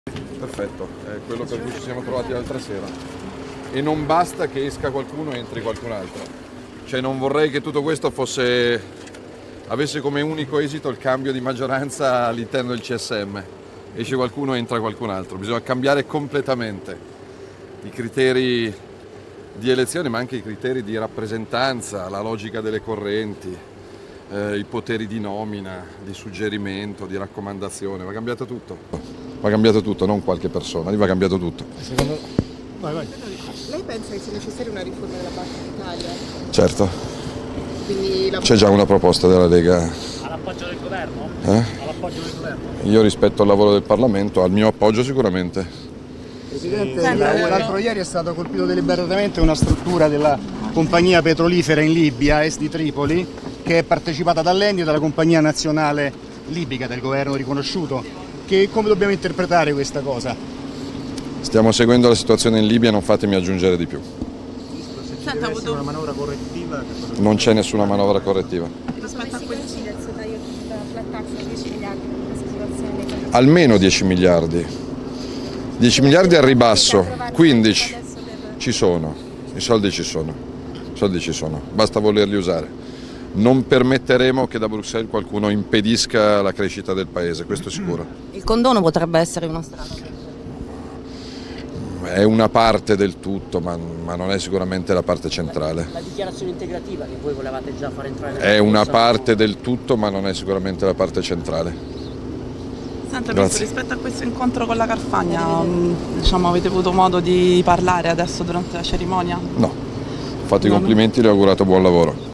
Conti pubblici, Salvini: "Non ci sarà nessuna manovra correttiva"
Lo ha detto a margine della cerimonia per il 245esimo anniversario della Gdf

POLITICA (Roma). "Non ci sarà alcuna manovra correttiva". Così  ha risposto il vicepremier e ministro dell'interno Matteo Salvini ai cronisti che gli domandavano della trattativa con l'Unione europea sulla procedura di infrazione a margine della cerimonia per il 245esimo anniversario della fondazione della Guardia di Finanza. "Prima vengono le garanzie per gli italiani e poi per tutti gli altri" ha aggiunto Salvini. Quanto a dove reperire le risorse e alla domanda se possano venire dal condono il ministro dell'Interno ha risposto che "è una parte del tutto ma non è sicuramente la parte centrale". (Gaia Romani/alanews)